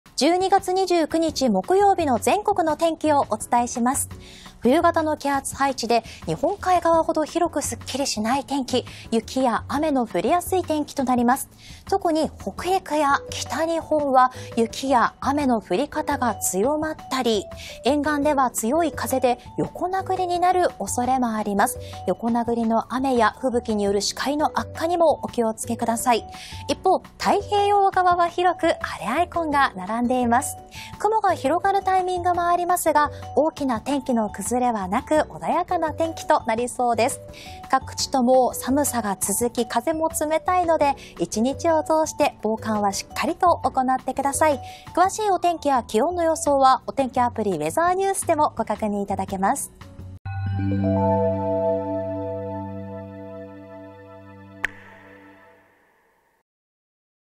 お天気キャスター解説 あす 12月29日(木)の天気
あす12月29日(木)の全国の天気を、ウェザーニュースキャスター山岸愛梨がお伝えします。

冬型の気圧配置になり、寒気が南下します。北海道や東北日本海側・北陸は雪や雨の降りやすい天気で、降り方が強まったり、雷やアラレを伴うおそれもあります。風も強いため、横殴りの雨や吹雪による視界の悪化にご注意ください。

一方、関東や東海、西日本太平洋側は日差しが届きます。雲の広がるところもありますが、大きな天気の崩れはなく広範囲で穏やかな天気となりそうです。

今日で仕事納めという方も多いのではないでしょうか。今年もお疲れ様でした。まだまだお仕事という方も、今年もあとわずかですね。1年頑張った自分を労ってあげてください。

美味しいご飯をたっぷり食べて、素敵な年末をお過ごしください。
それでは。